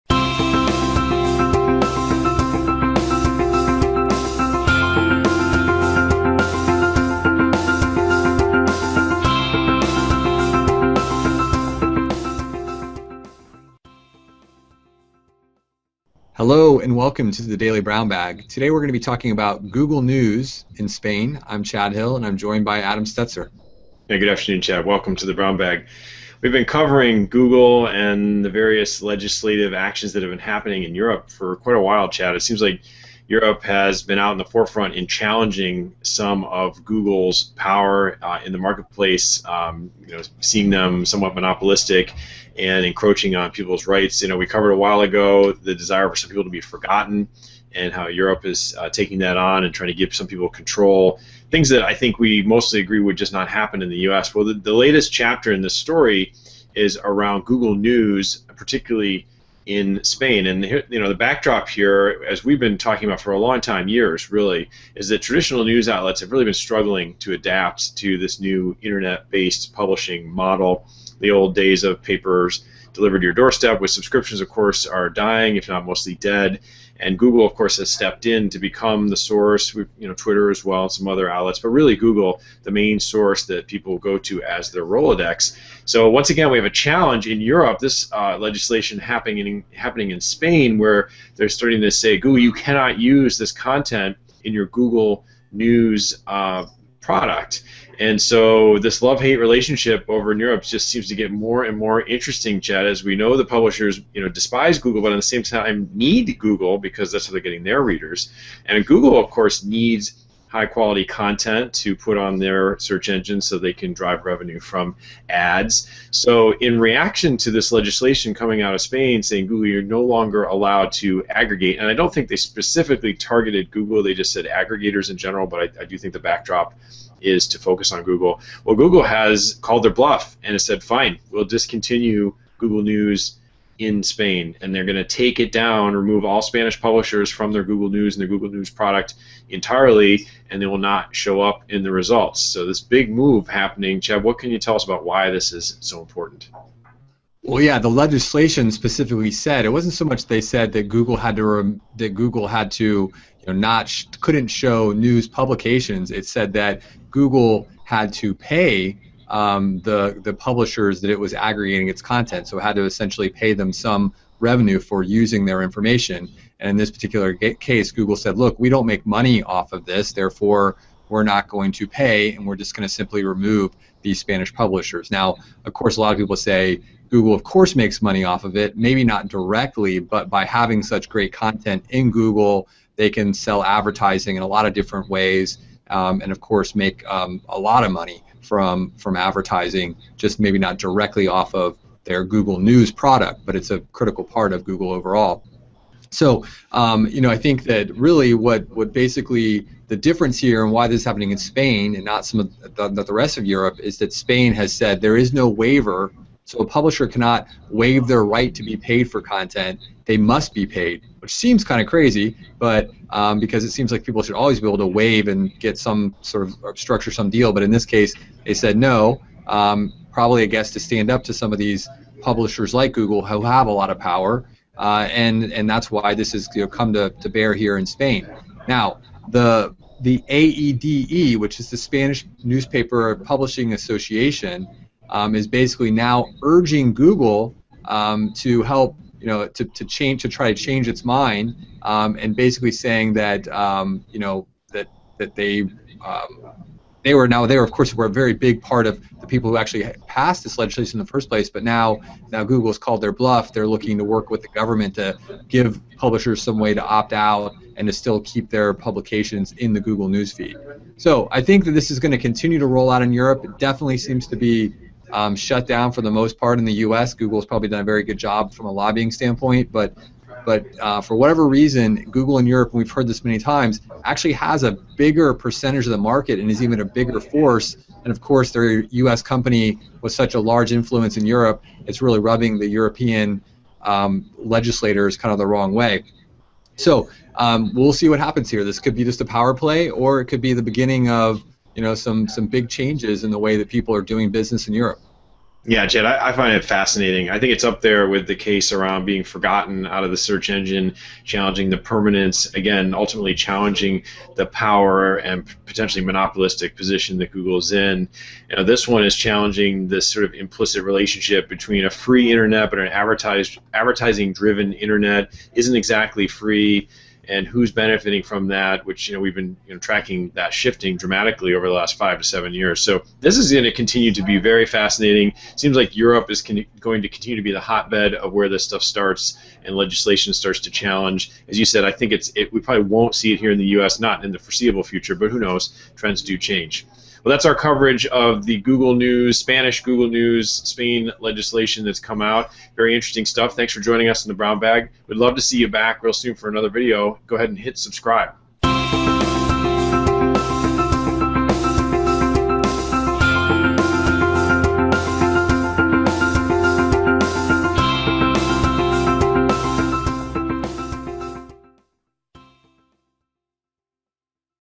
Love-Hate Relationship Between Spanish Publishers & Google News
Once again, there was a challenge in Europe (particularly in Spain) that concerned publishers and Google News. In Spain, there was recent legislation saying that Google could no longer aggregate news publications, leading to Google simply removing all Spanish publishers from their Google News product entirely. Watch this Daily Brown Bag to learn more about this love-hate relationship over in Europe (in this case, involving Spanish publishers) and its impact on the Google News product.